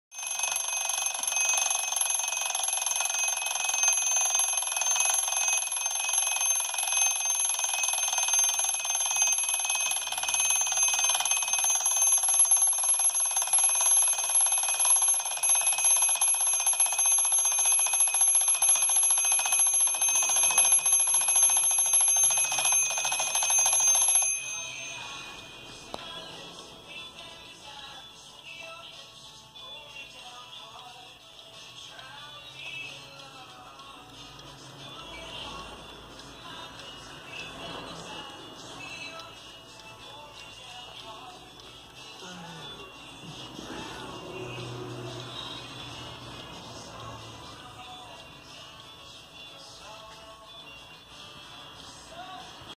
Relógio despertador marca Herweg cor preta
Mais um modelo de despertador pra tirar o povão da cama na mesma hora.